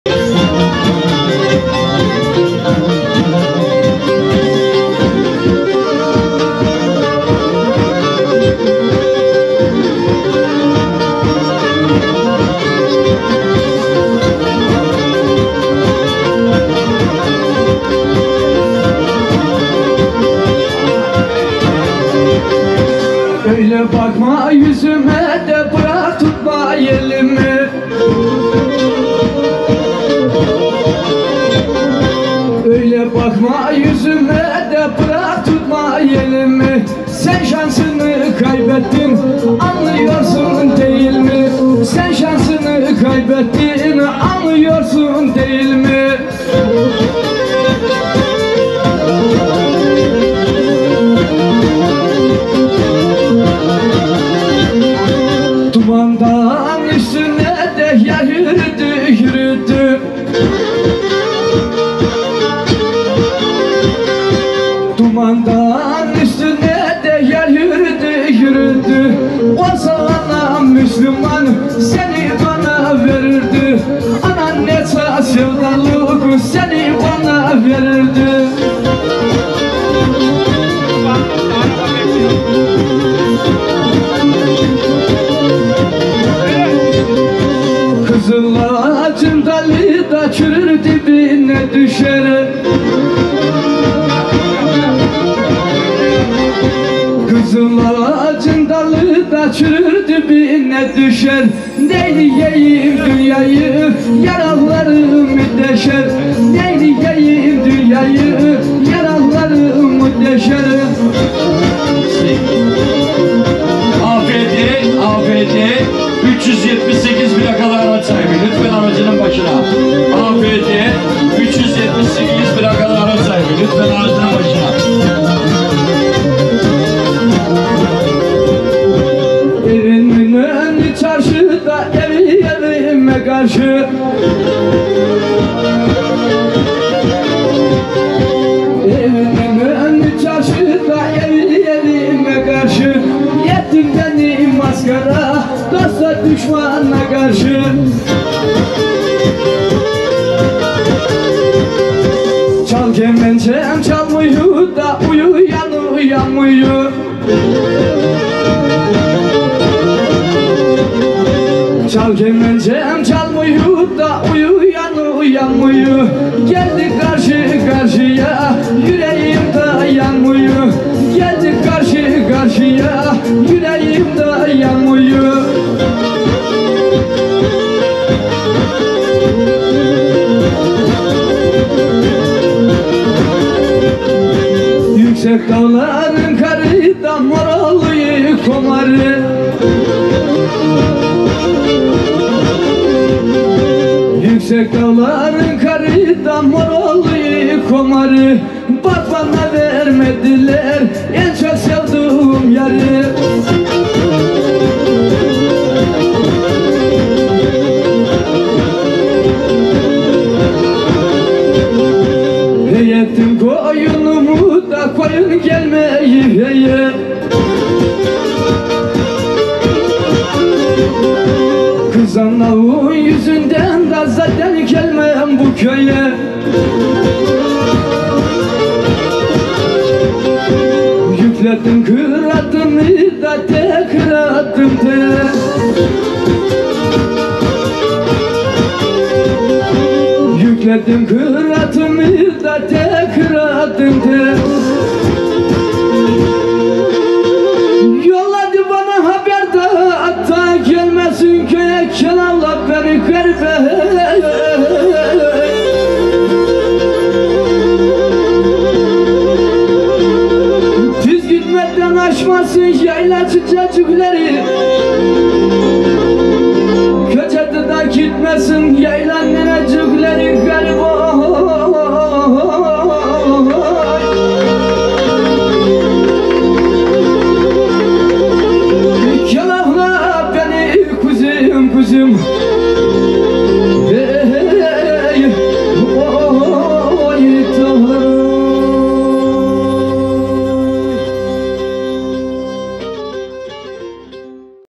Enes Akyüz - Fengo Yayla Şenlikleri - 2023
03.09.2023 Düzköy Aykut Mahallesi - Fengo Yayla Şenliklerinde çekilmiştir.